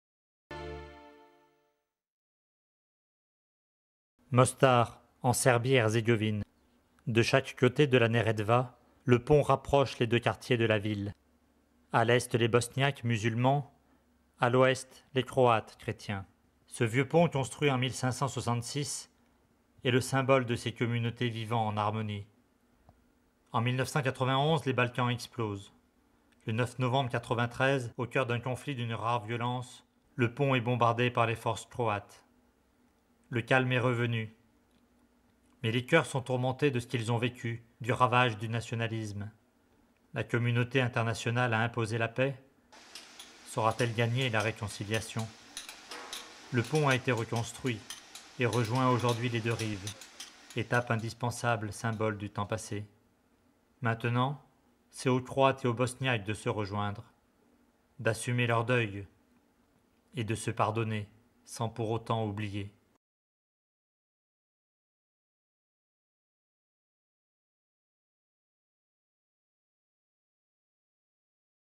Mostar Bosnie Herzegovine 2006
L'histoire du pont de Mostar, en Bosnie Herzégovine, racontée en 1 minute.